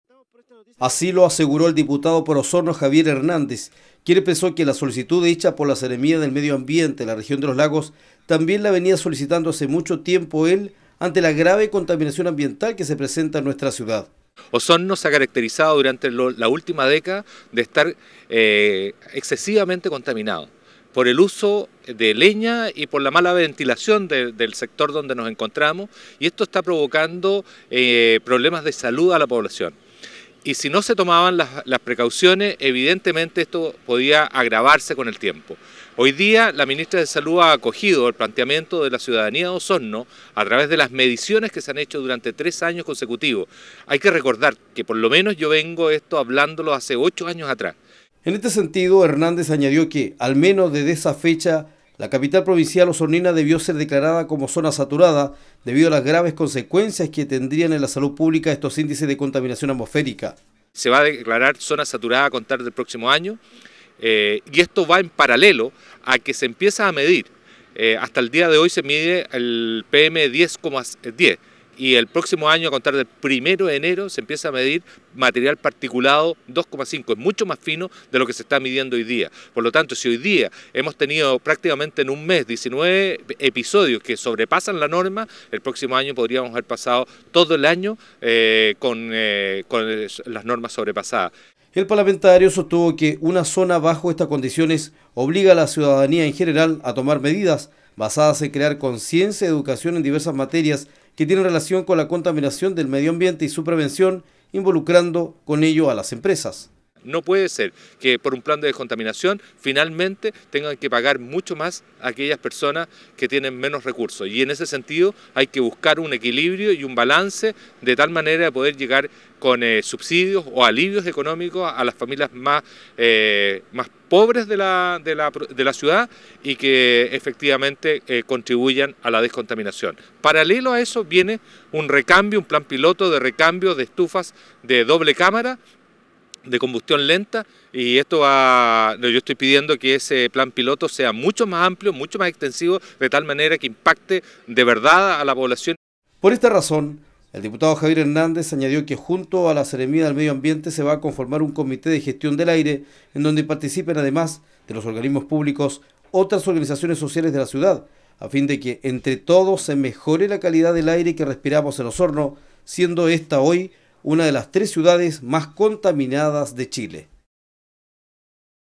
POR ALTOS NIVELES DE CONTAMINACIÓN
Declaran a Osorno como zona saturada debido a la mala calidad del aire que presenta la comuna, al igual que ciudades como Valdivia y Temuco.